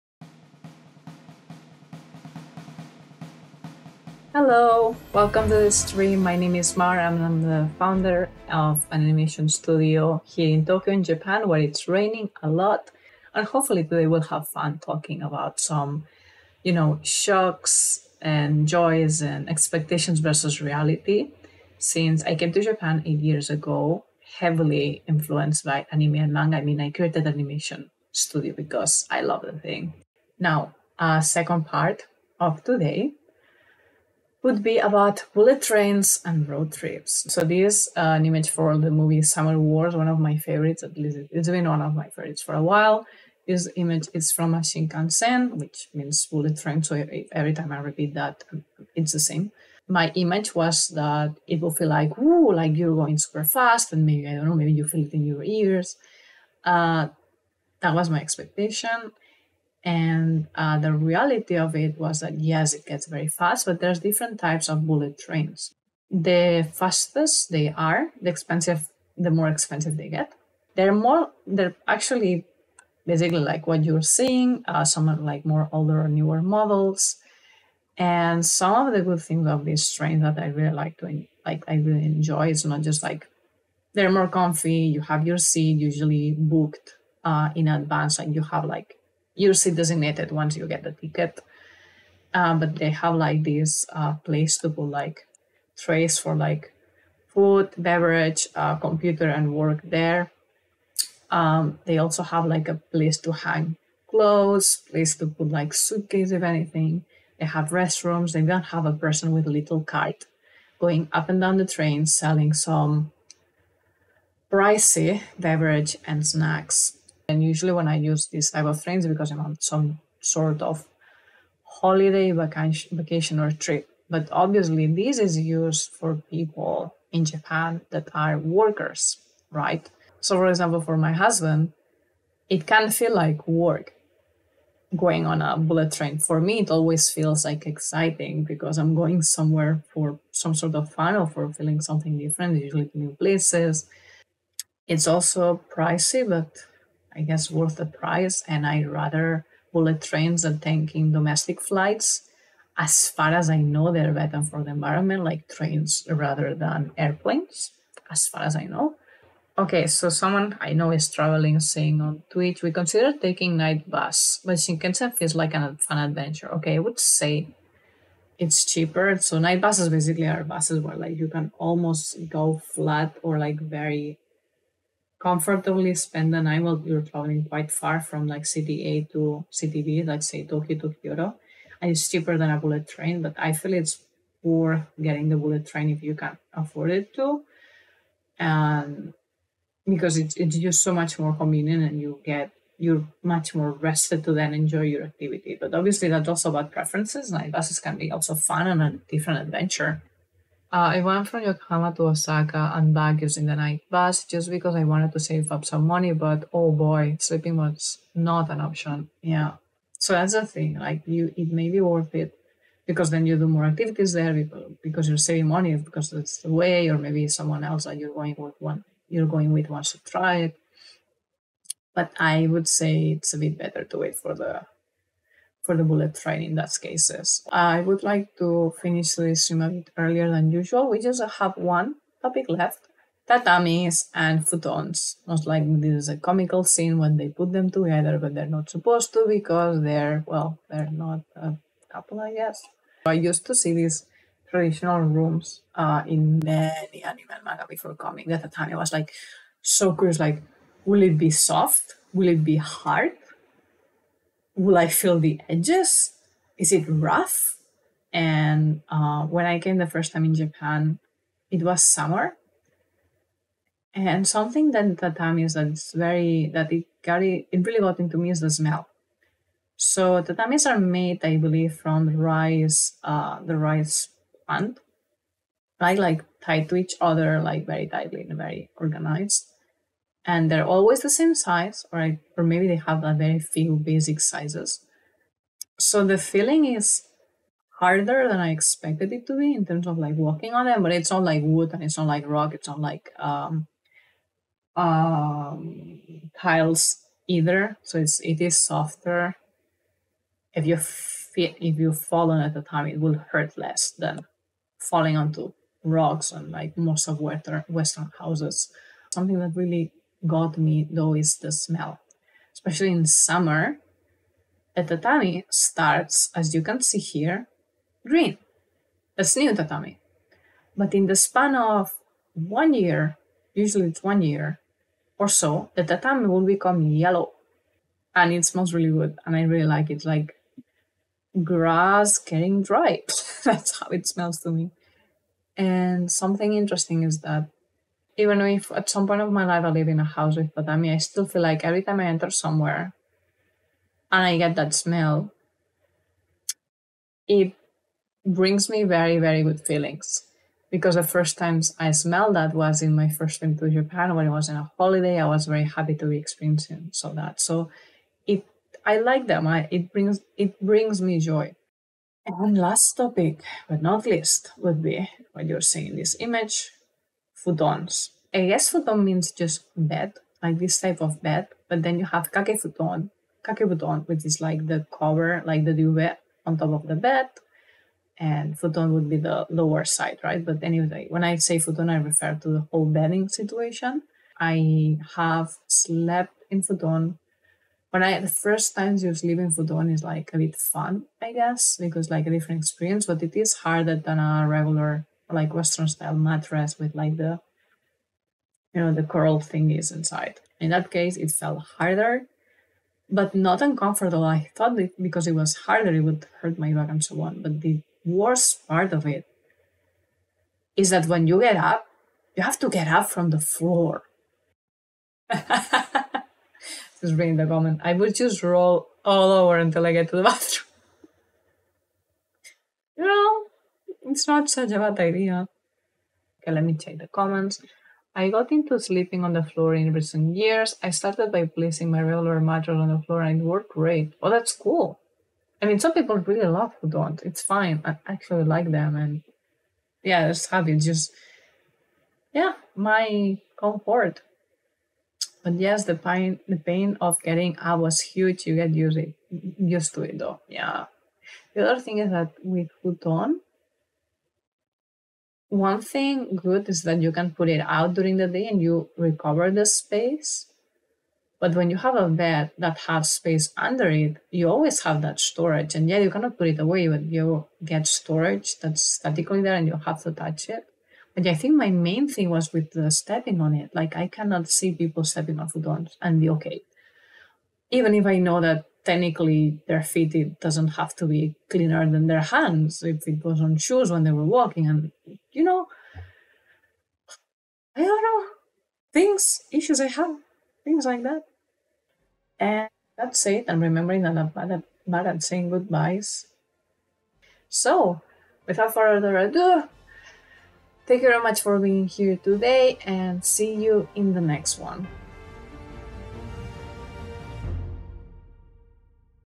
Futon, Tatamis and BULLET trains - My joys and shocks in Japan
Fiction vs Reality. This video is part of a SERIES sharing my experience living in Japan as an anime fan. This edition is all about transport, with some little tatami talk here and there. Why does shinkansen always feels so exciting for me and so dreadful for others? And night buses… let’s talk about dreadful D:

*Timestamps*
00:00 Intro
00:30 Bullet Trains!
02:39 Night buses
03:47 Tatamis
06:01 Futons
08:11 Otro

This video is an excerpt from our 2023-June-02 livestream.